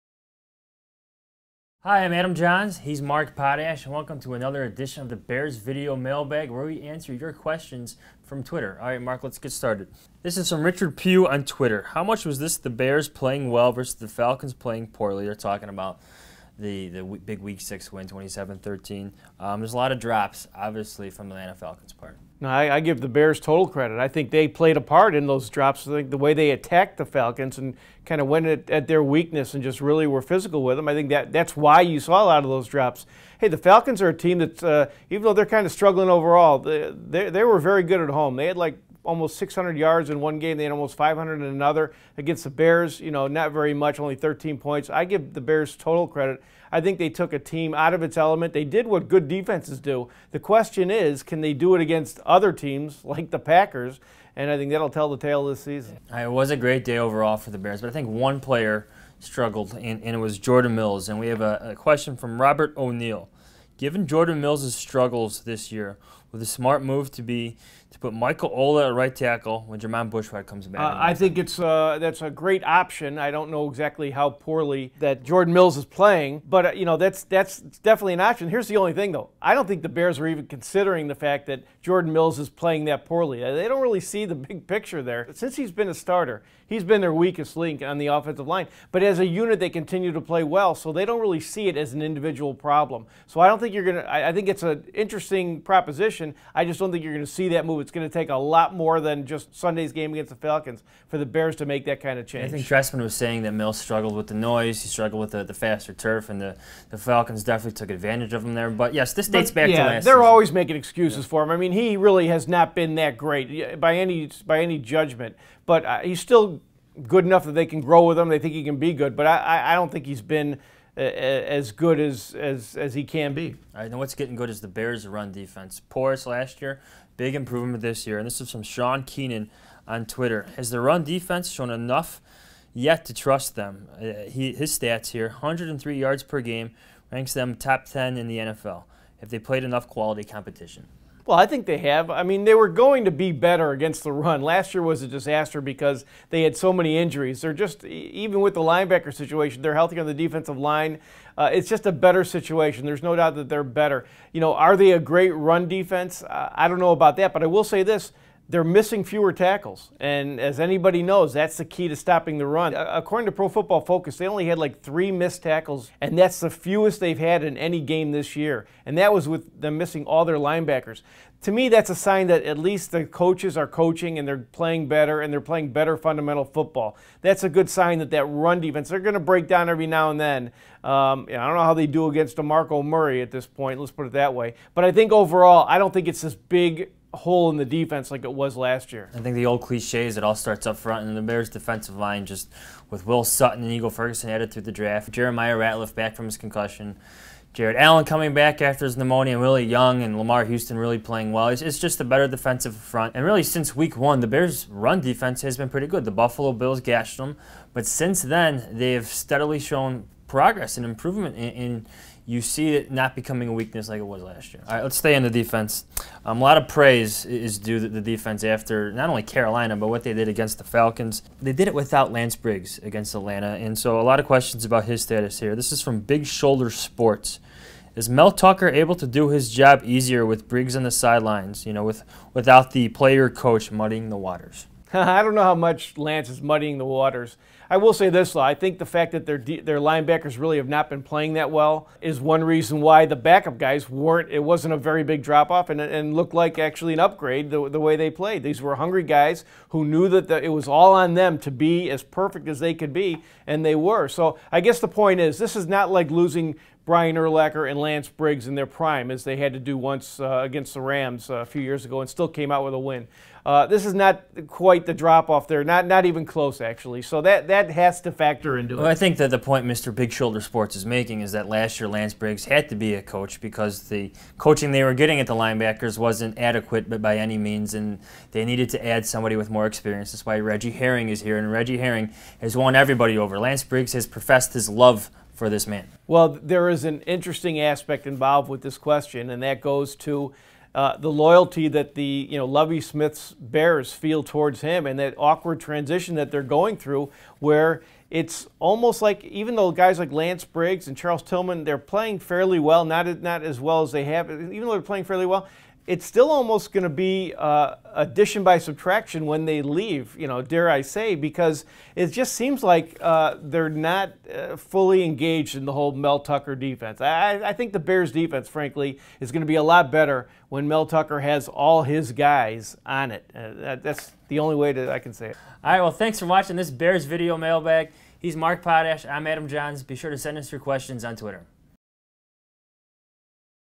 Bears Mailbag for October 15, 2014
Adam Jahns and Mark Potash of the Chicago Sun-Times answer your pressing Bears questions via Twitter, this week talking about rookie linebackers, the run defense, Jordan Mills, Lance Briggs and more.